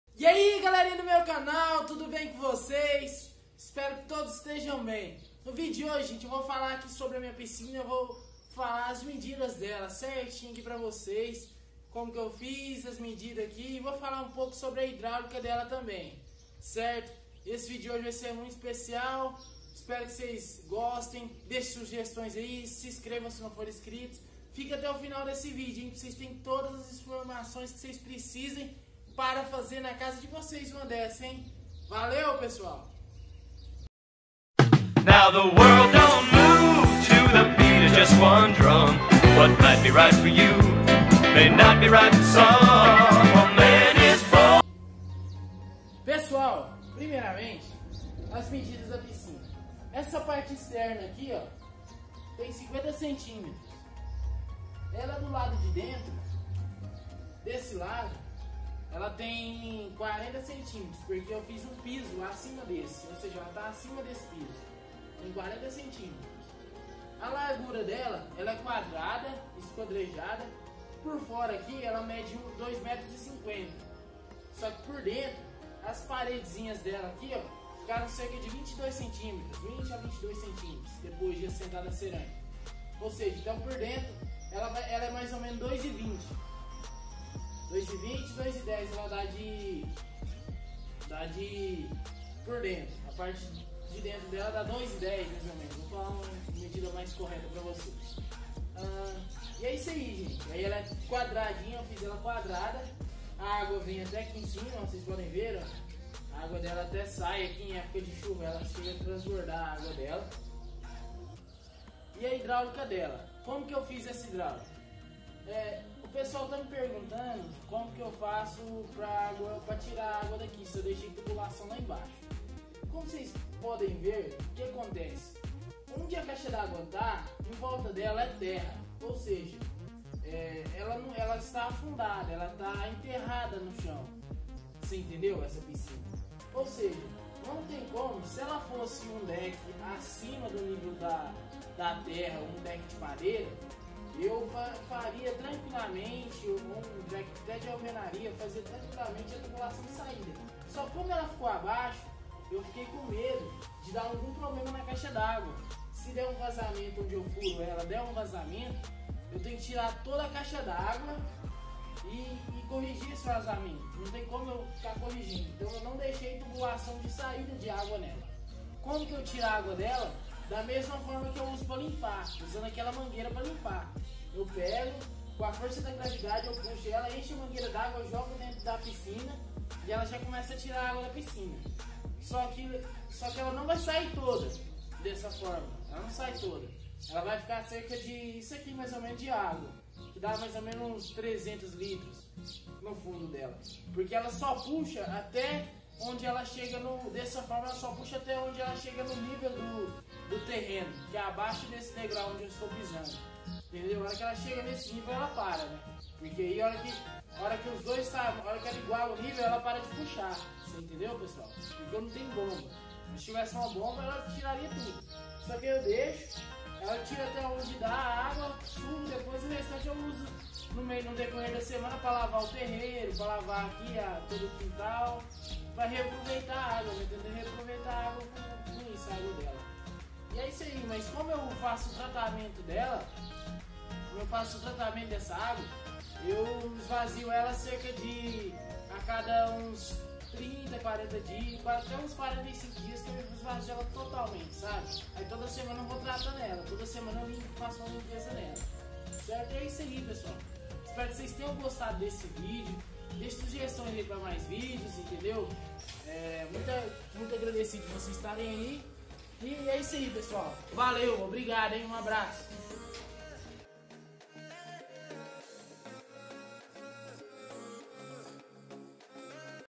PISCINA de CAIXA D 'ÁGUA !!! MEDIDAS E PARTE HIDRÁULICA.
Neste vídeo mostro as medidas da piscina de caixa dágua e algumas informações cruciais para fazerem uma dessa na casa de vocês .
OBRIGADO !!!